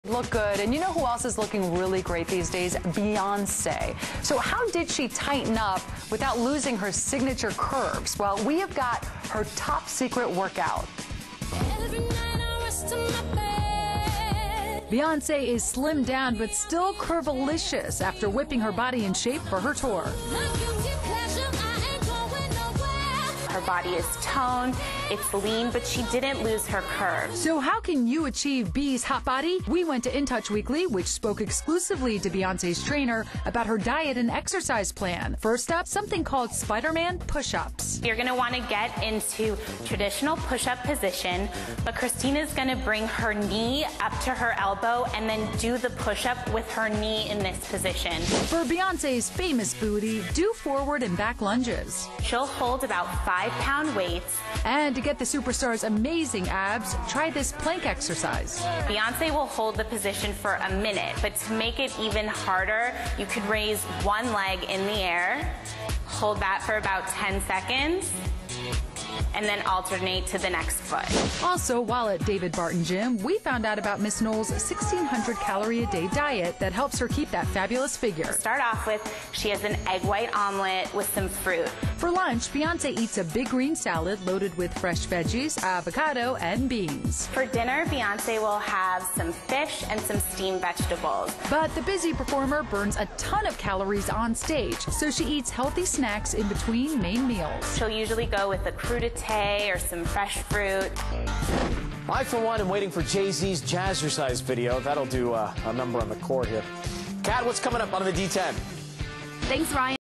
Beyonce's Workout
Beyonce's trainer shares her workout tips with In Touch Weekly's Lifestyle Editor Amy Silverberg